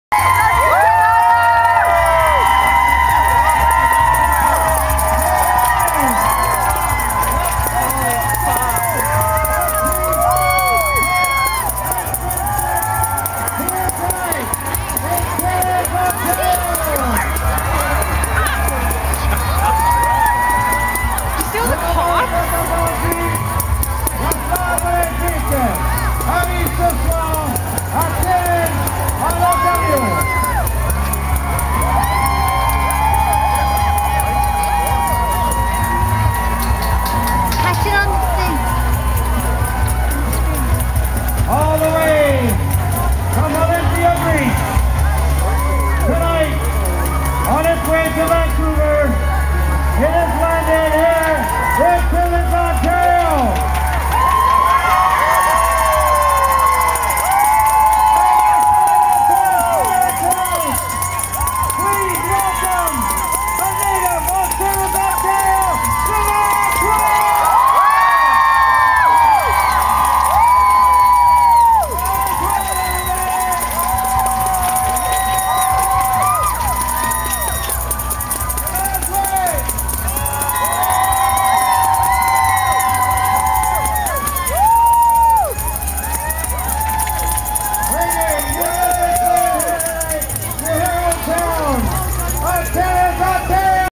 Shania Twain & The Olympic Torch - Timmins, ON
Shania Twain, with the Olympic Torch :)

Hollinger Park, Timmins ON Canada. Jan. 1st 2010